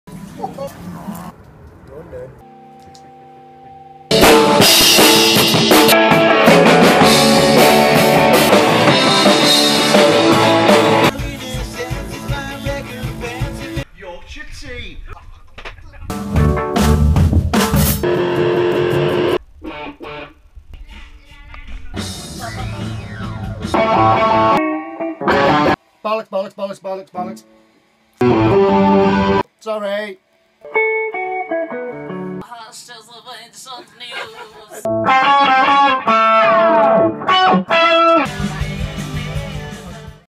EP1 - Recording at Hackney Road
The Tuesday Club at Hackney Road Studios

Steve Honest at the helm.  Fueled by fat boy breakfast and builders tea.